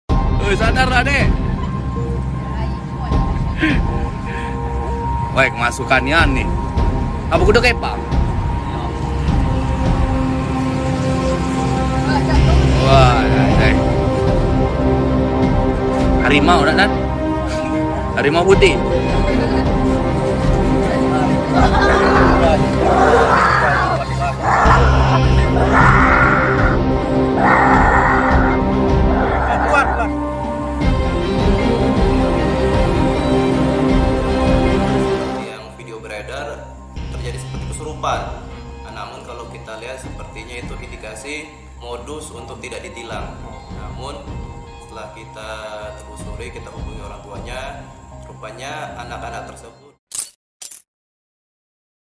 Hendak Ditilang Polisi, Seorang Remaja Kesurupan Siluman Harimau
Seorang remaja yang terkena tilang oleh Polantas tiba-tiba mengalami kesurupan siluman harimau. Polisi yang melihat tingkah pelanggar yang terjaring dalam razia operasi zebra tahun 2022 tersebut terlihat tenang meski menghadapi orang yang tengah kerasukan makhluk halus. Bahkan dalam video juga terlihat teman pelaku yang ikut terjaring hanya tersenyum melihat hal yang terjadi. Kasat Lantas Polres Lebong, IPTU Teguh Prasetyo menyatakan bahwa kejadian tersebut dilakukan hanya untuk modus agar terlepas dari tilang.